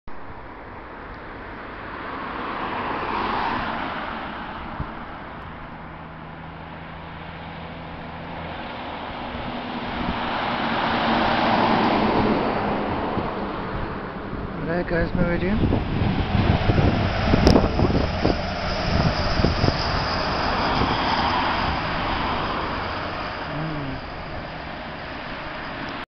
EMR Class 220 'Meridian' through Sundon Park 4
Here we see another of the Class 220 trains in the EMR [East Midland Railway] purple and silver livery.
But this time, it is going north rather than south.
They are rather popular.